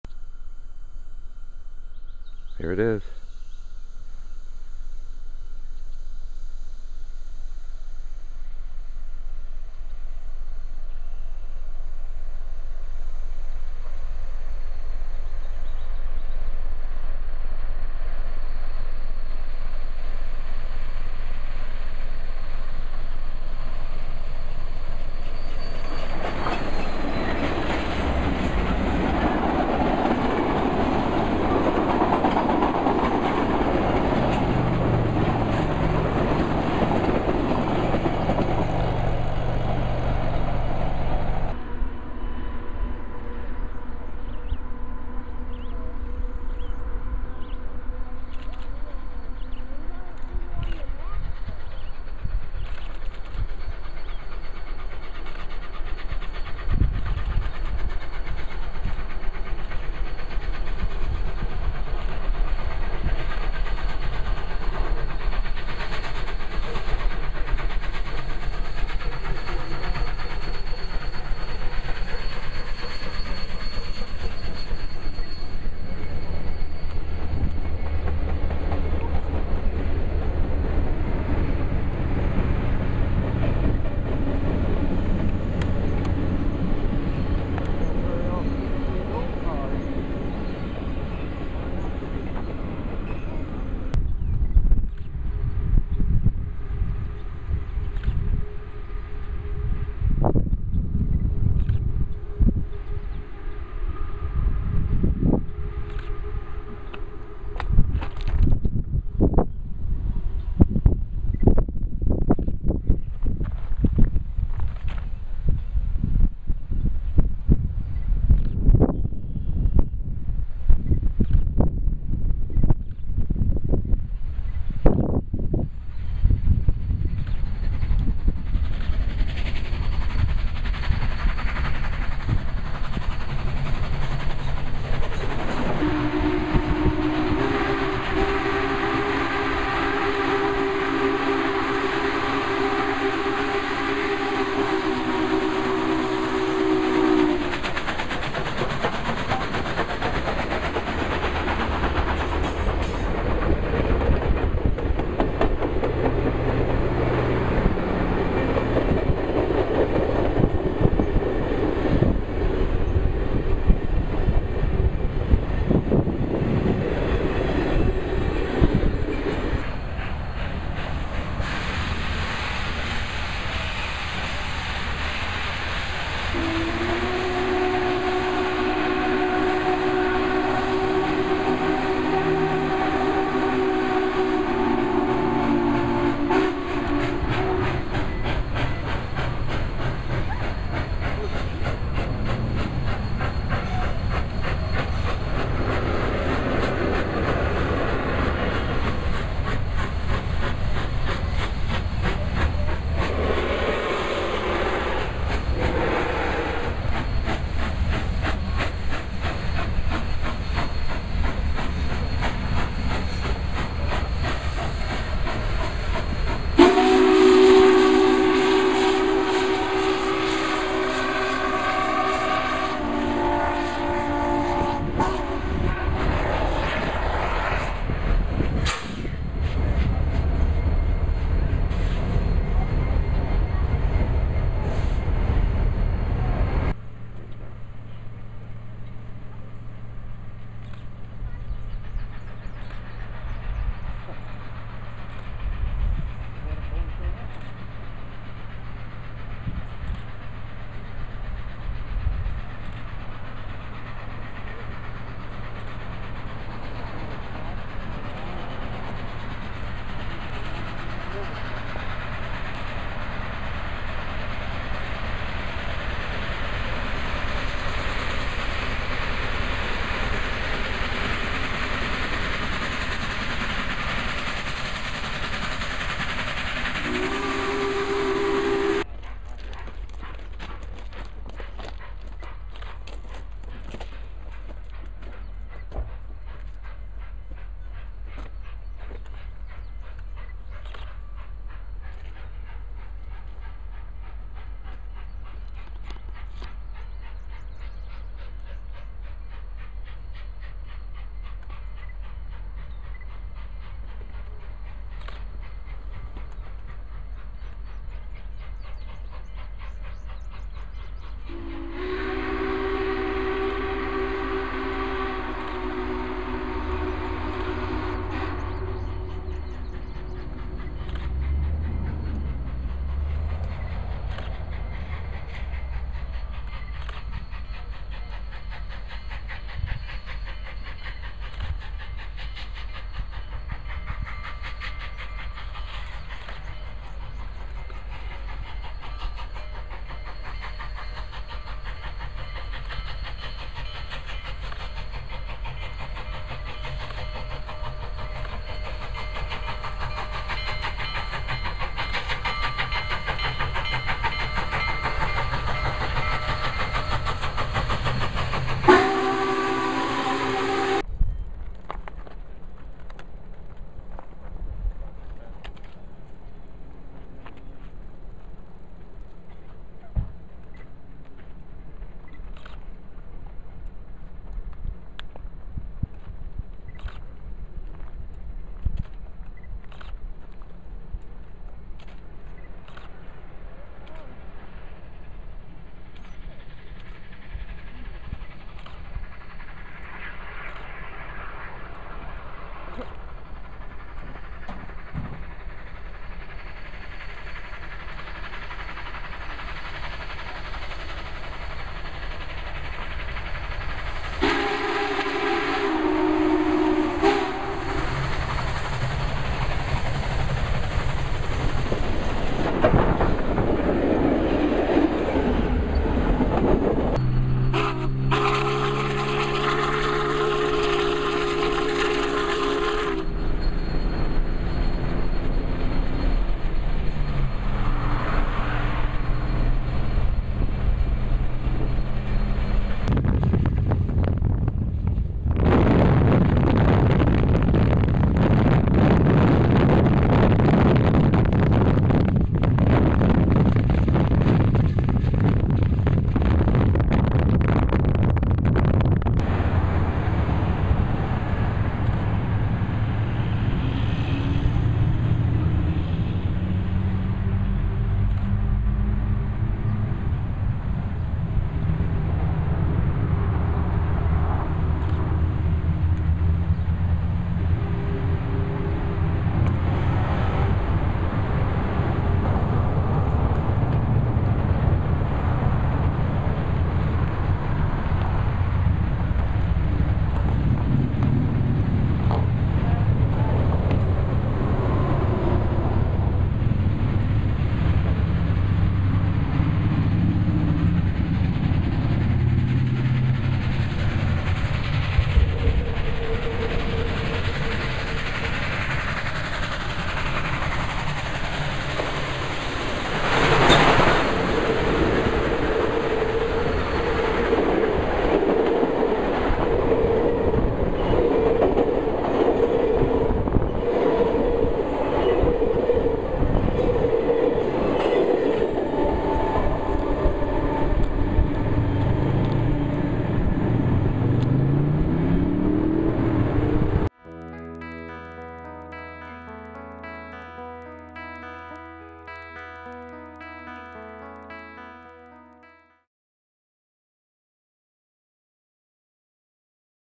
Union Pacific #844 Little Rock Excursion
Various clips from the Missouri & Illinois legs of the UP 844's Little Rock Excursion in June 2011.